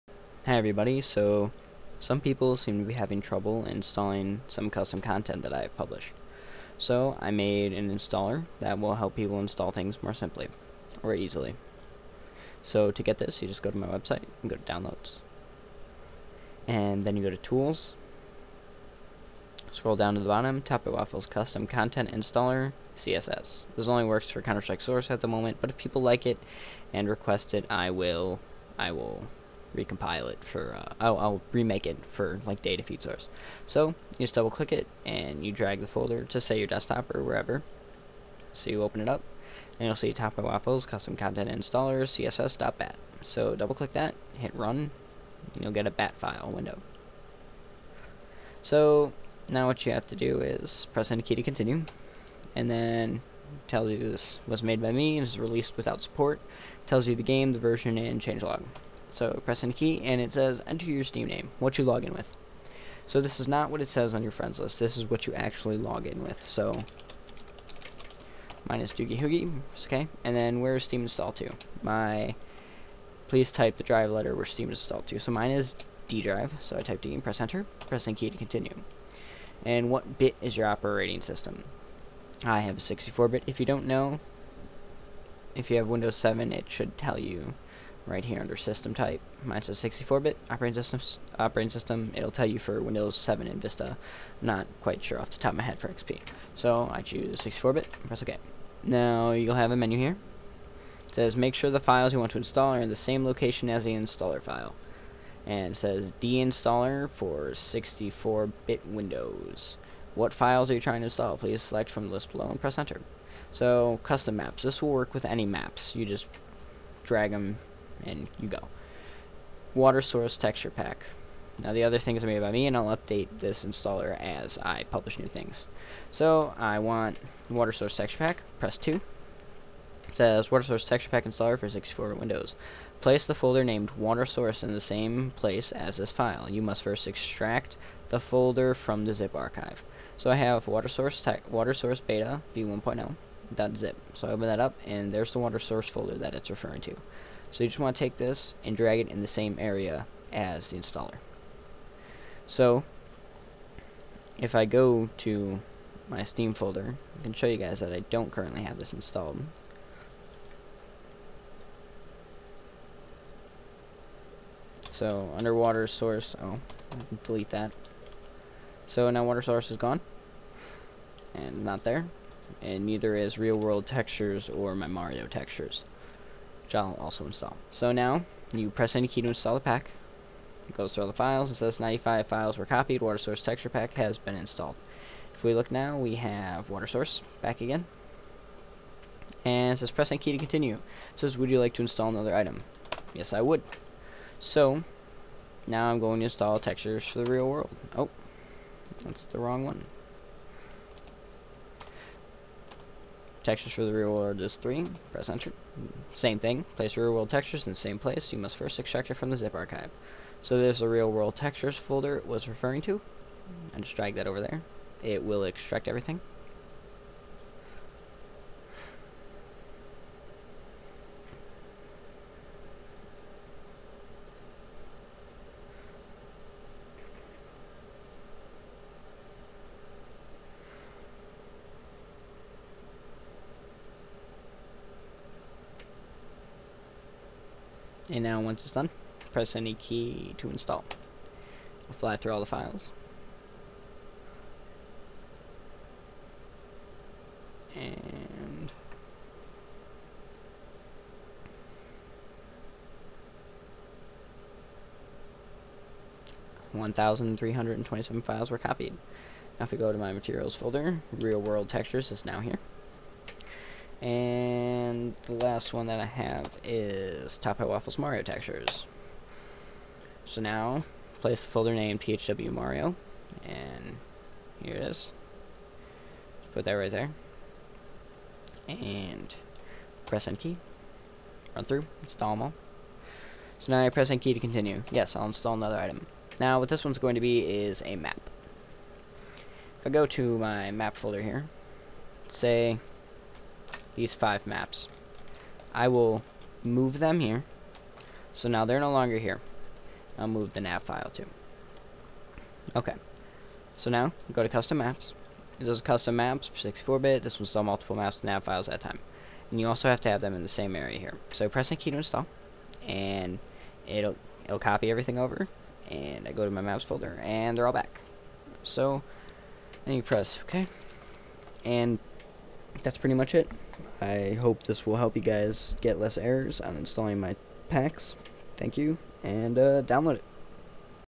TopHATTwaffle's Custom Content Installer CS:S
***This now works for most source engine games!!1***
Sorry for bad audio quality! This will install custom maps, and most of my custom content.